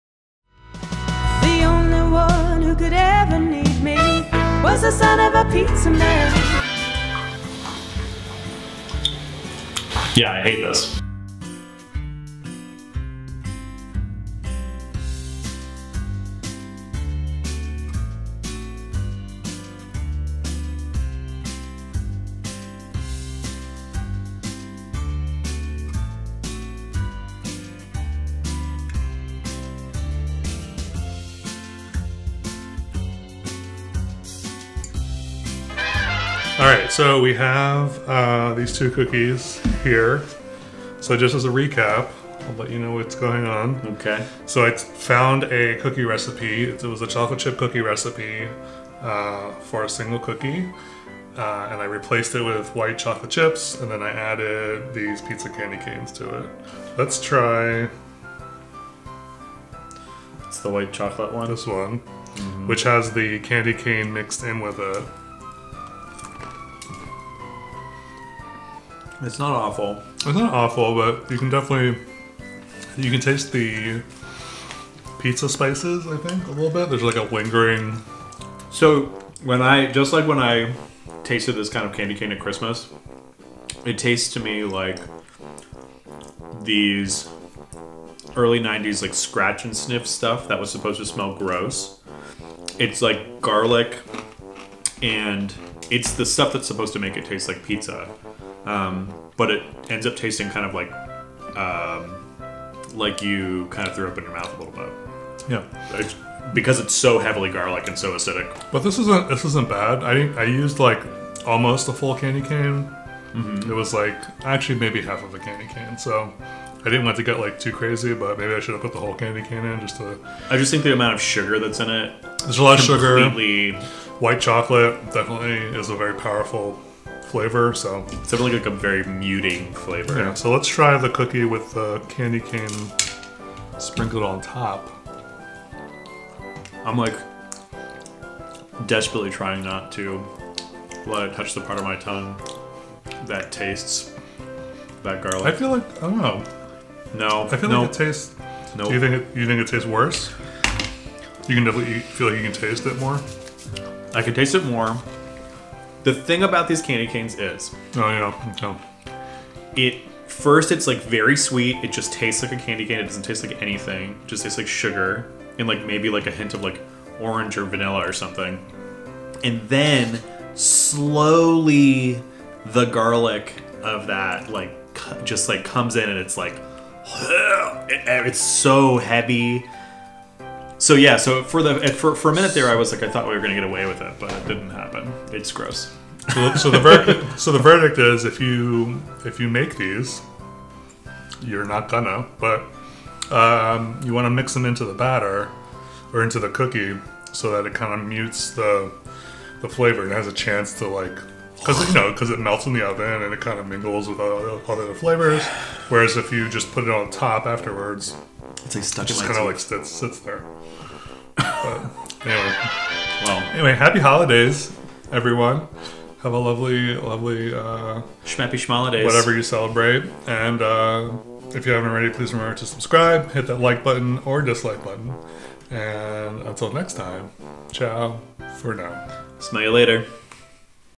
Pizza Candy Cane Cookies
I was curious about what a pizza-flavored candy cane would taste like in a cookie. Watch to find out the results!

BECOME PART OF THE FAMILY!

LET'S GET SOCIAL!

CREDITS!

Photography courtesy of:
Kedzie Photography